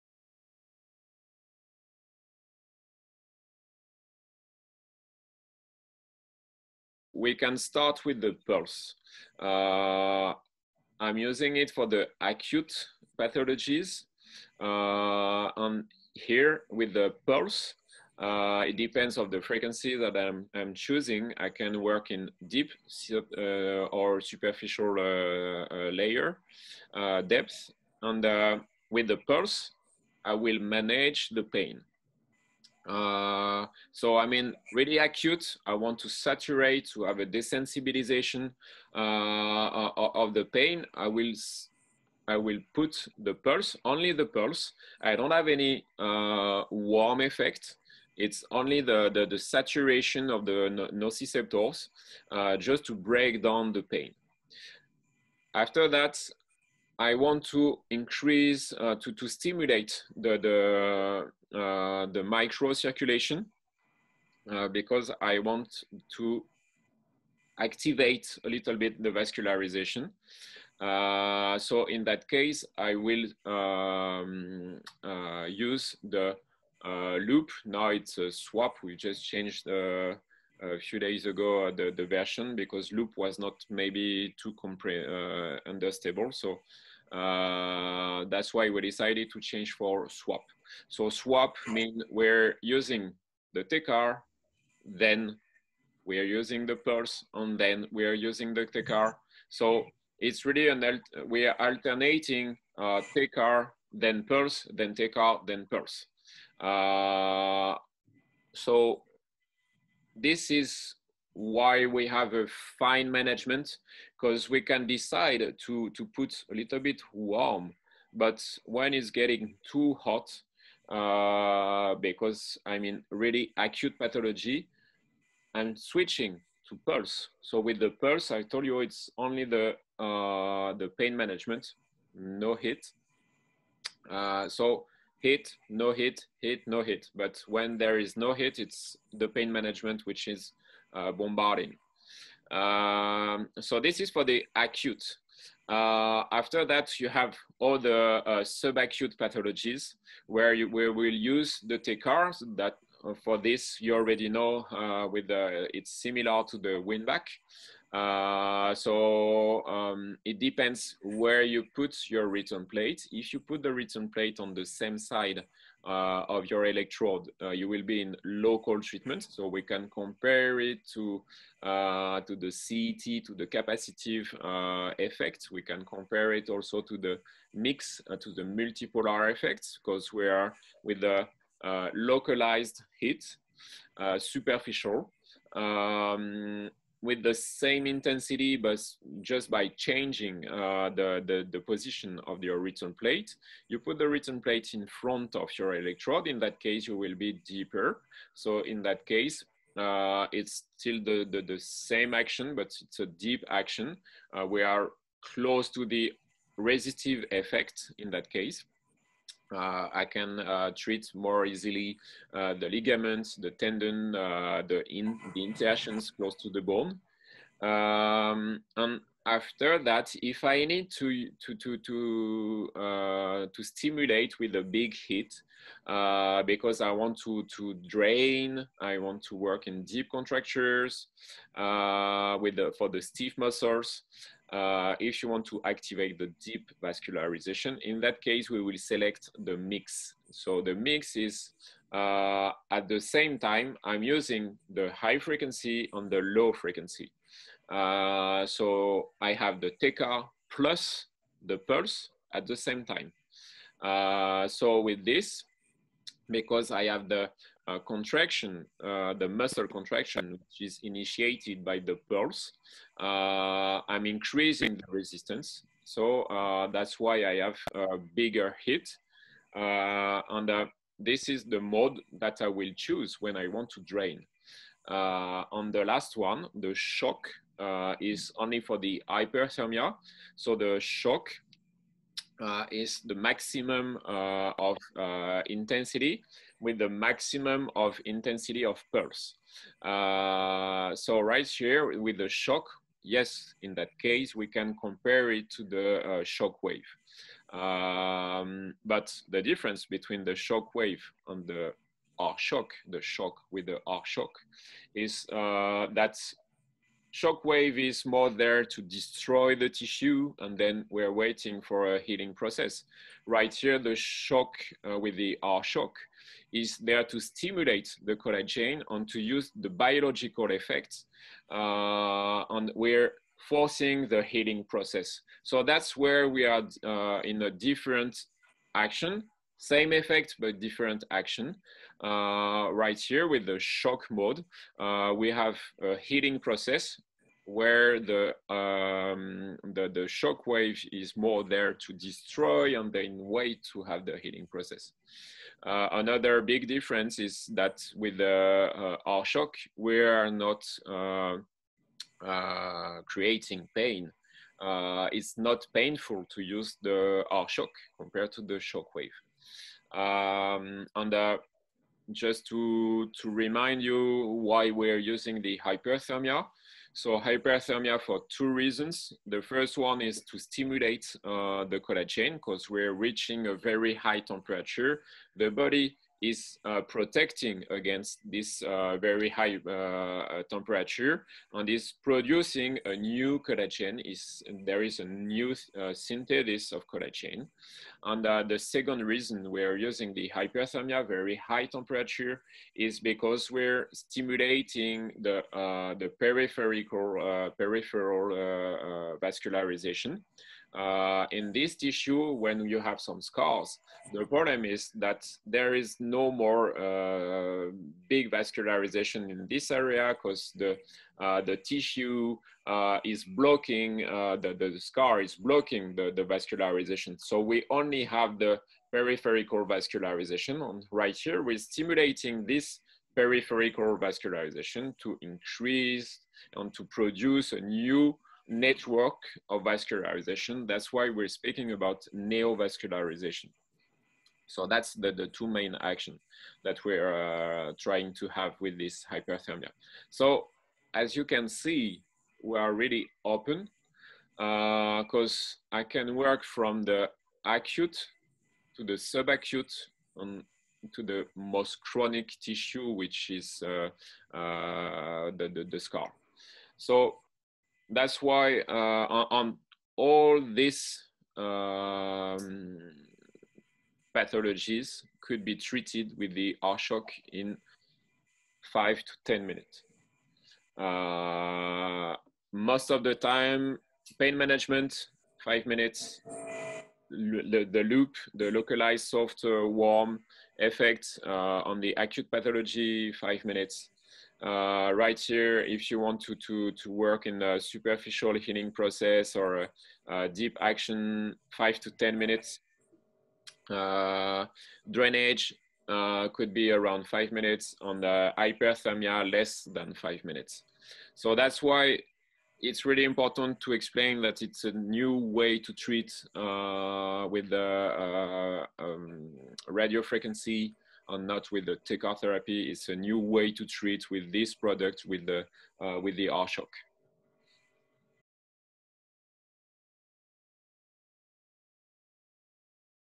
R Shock - Targeted effects
The notion of depth will be optimized by the placement of the return
electrode

- Superficial action: return electrode on the same side as the emitter  electrode

- Deep action: return electrode positioned in front of the emitter electrode